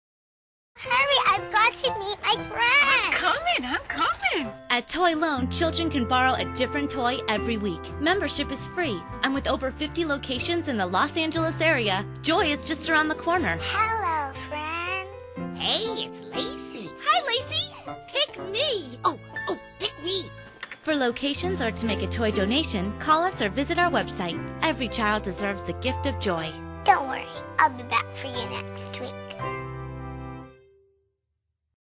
Toy Loan Program - Public Service Announcement - Santa Monica CityTV - PSA
This public service announcement was produced for the County of Los Angeles Toy Loan Program which is celebrating its 75 anniversary. A volunteer creative team put together this high quality PSA. We are honored to have collaborated with the LA County Channel to create this PSA. August 6th is when we will find out who wins the Emmy Award! Our congratulations goes to Deborah Read, Director; Emily Butali, Producer; Renee McClellan, Writer and Sheila House, Associate Producer.